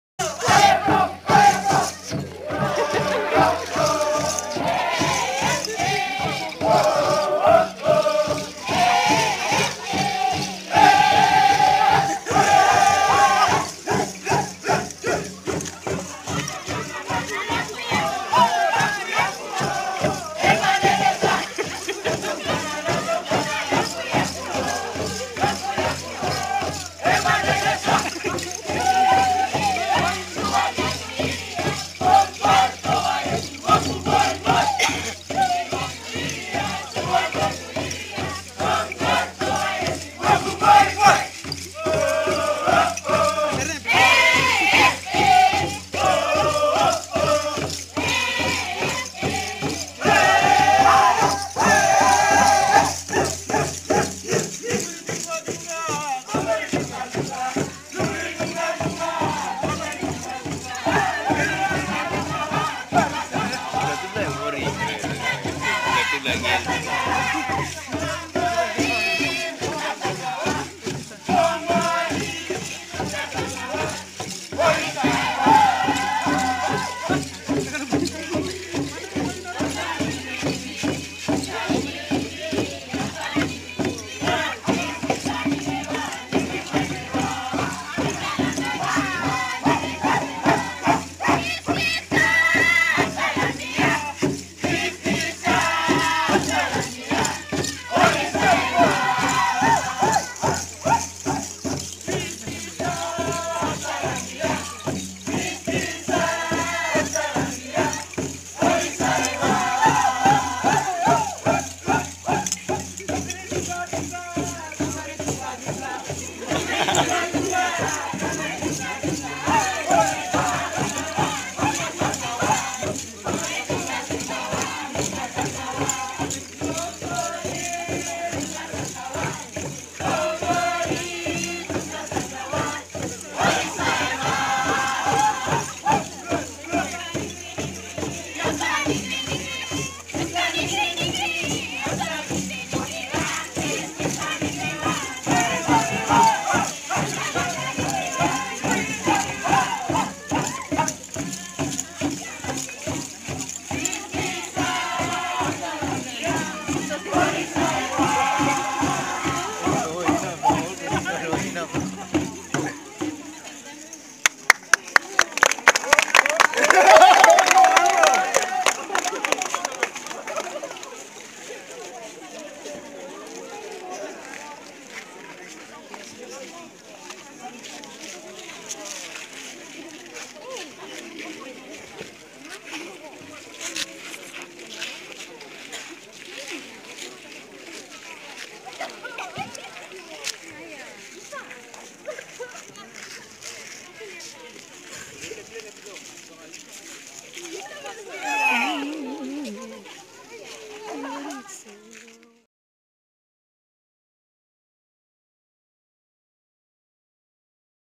Divine Word University-SBC-2019
Powerful student teachers of west sepik province, dancing to the beat of kundu drums.